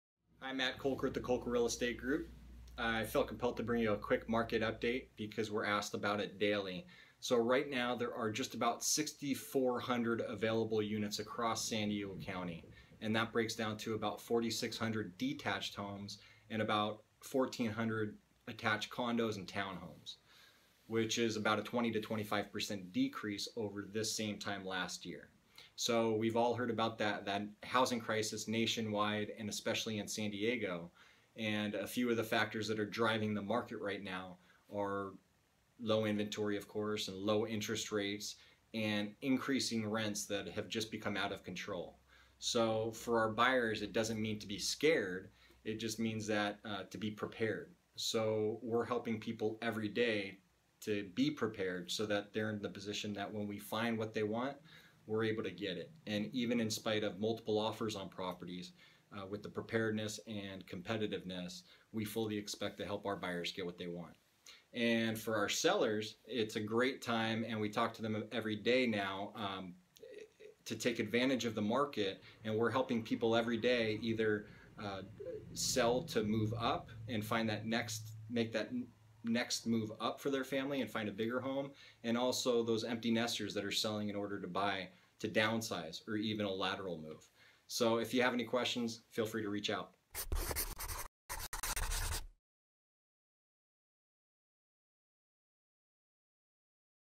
End of July Market Update | Matt's Minutes
Matt Kolker gives us a realistic idea of what's going on in the market right now in San Diego.

**NOTE ABOUT NUMBERS** If you're thinking that some of the numbers don't add up, you are right! Matt left out the mobile and manufactured homes included in the total inventory to give a lower number of available attached and detached homes.

*PS* These numbers change DAILY. To get the latest information, give us a call!